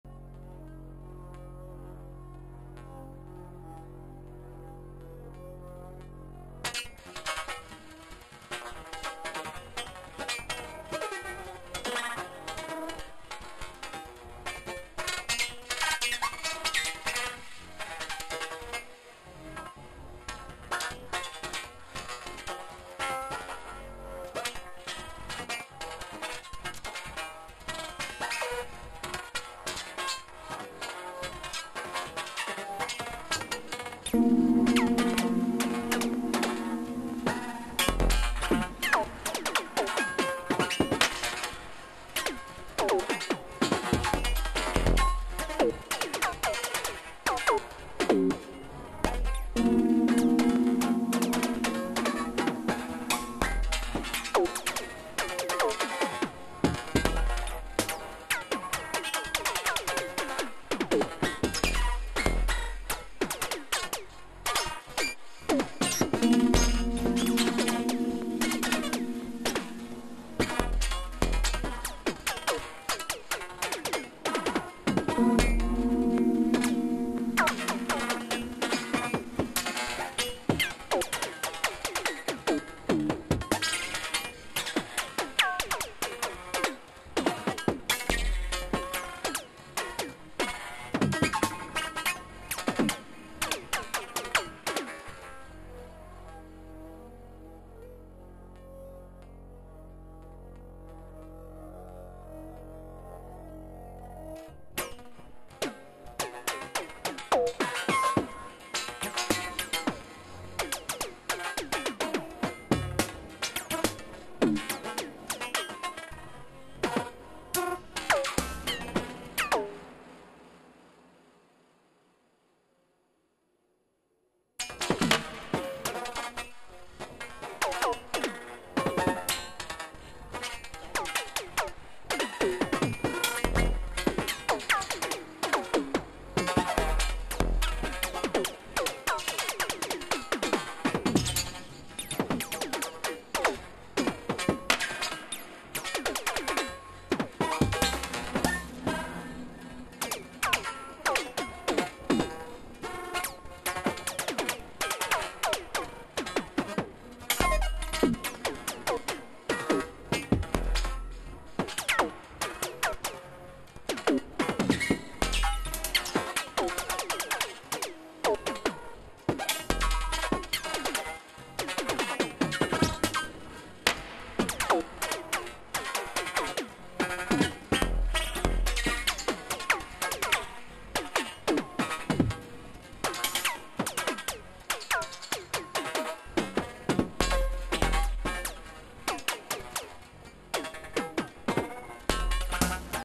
SurgeXT VCV Rack2 IDM Generative patch
SurgeXT @VCVRack IDM Generative patch + Drumcomputer + SandmanPro // sugarbytes unfilteredaudio vcvrack surgext .